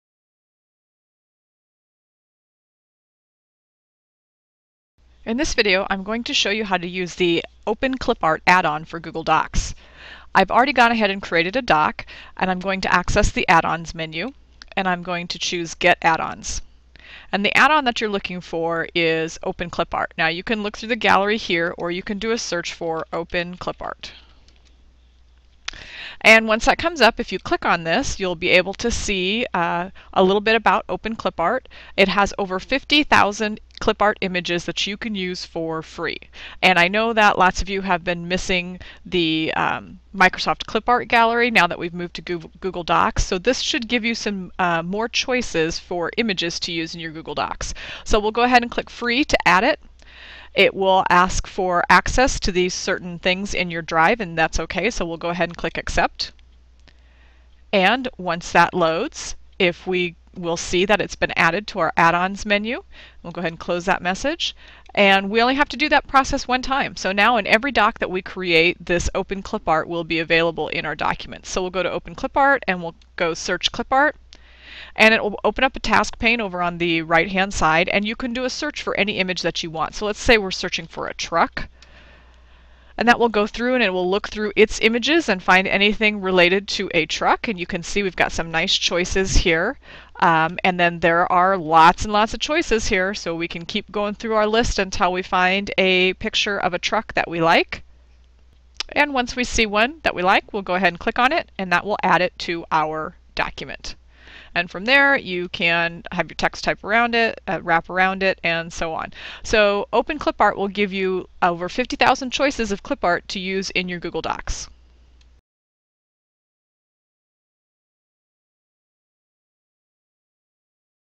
OpenClipart Add-on for Google Docs
This video will show you how to use the OpenClipart add-on for Google Docs.